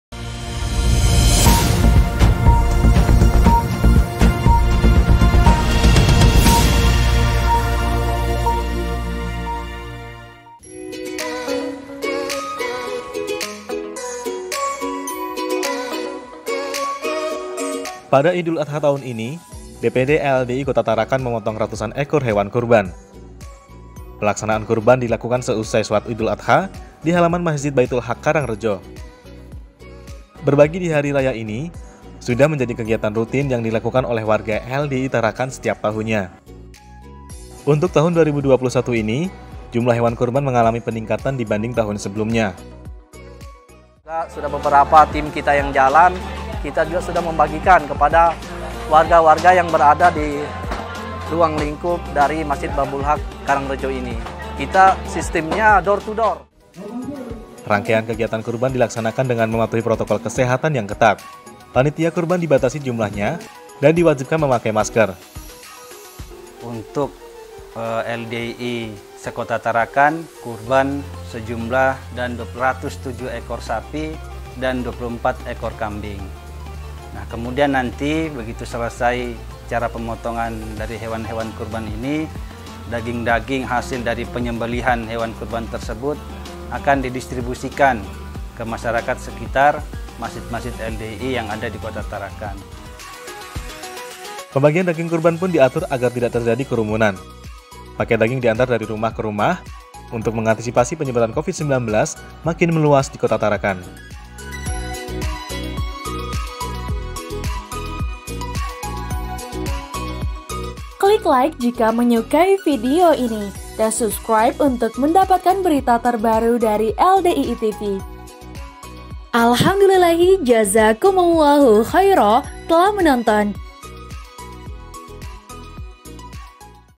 LDII TV: DPD LDII Tarakan Potong Ratusan Hewan Kurban
Pada Idul Adha tahun ini DPD LDII Kota Tarakan memotong ratusan ekor hewan kurban. Pelaksanaan kurban dilakukan seusai sholat Idul Adha di halaman masjid Baitul Haq Karang Rejo.

Berbagi di hari raya ini sudah menjadi kegiatan rutin yang dilakukan warga LDII Tarakan setiap tahunnya. Untuk tahun 2021 ini jumlah hewan kurban mengalami peningkatan dibanding tahun sebelumnya.

Facebook : Lembaga Dakwah Islam Indonesia